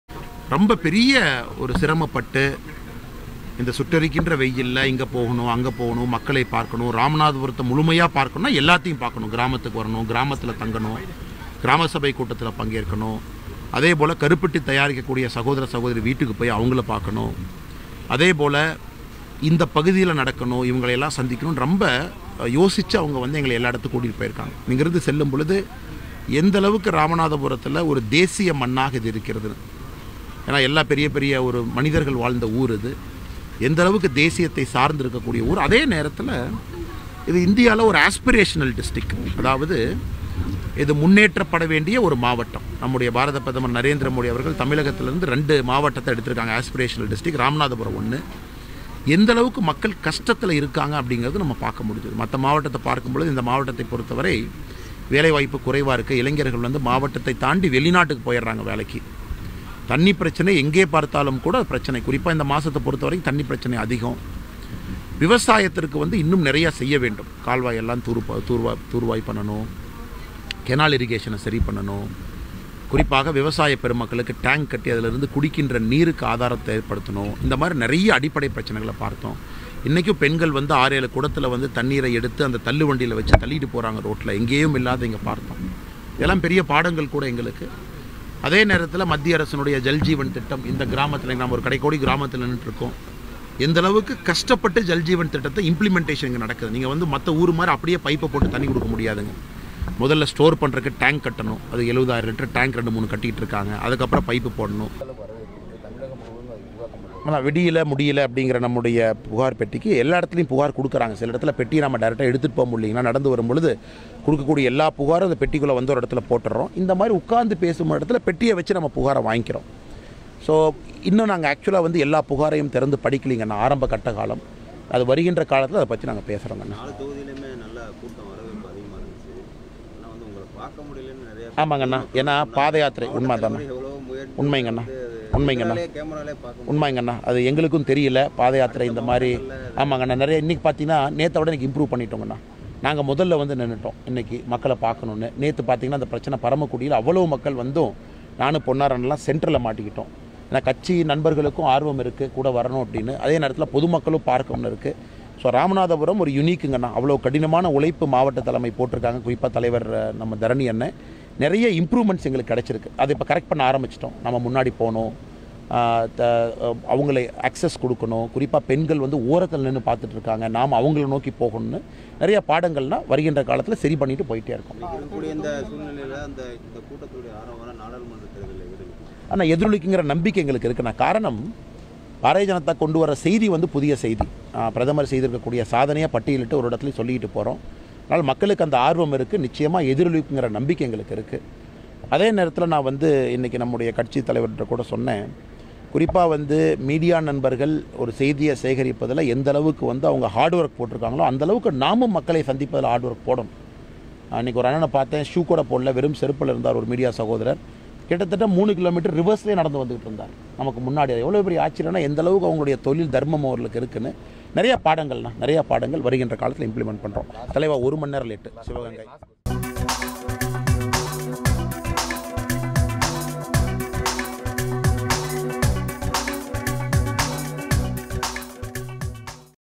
" பிரதமர் மோடியால் தேர்ந்தெடுக்கப்பட்ட மாவட்டம் ராமநாதபுரம்.. "அண்ணாமலை போடும் கணக்கு
" பிரதமர் மோடியால் தேர்ந்தெடுக்கப்பட்ட மாவட்டம் ராமநாதபுரம்.. " இது ஒரு தேசிய மண்.. பெரிய பெரிய மனிதர்கள் வாழ்ந்த ஊர் இது.. அண்ணாமலை போடும் கணக்கு